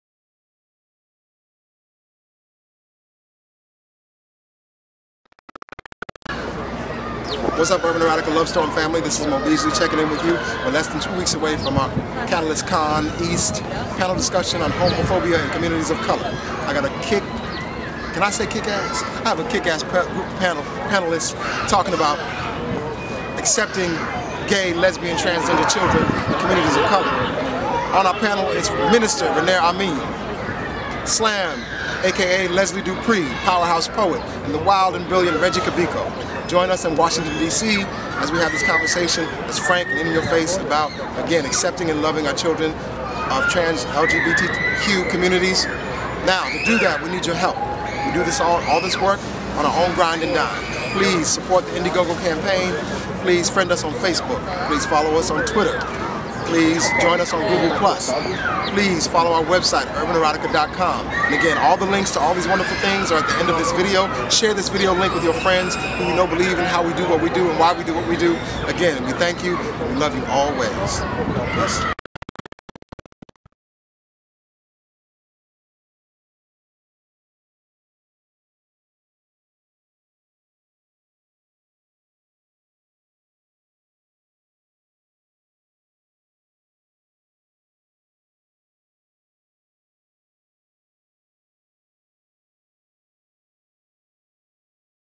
Mo Beasley, the "Headmaster" at CatalystCon D.C.
Mo Beasley promotes his "Pride & Prejudice" panel on Homophobia in communities of color CatalystCon East 2013.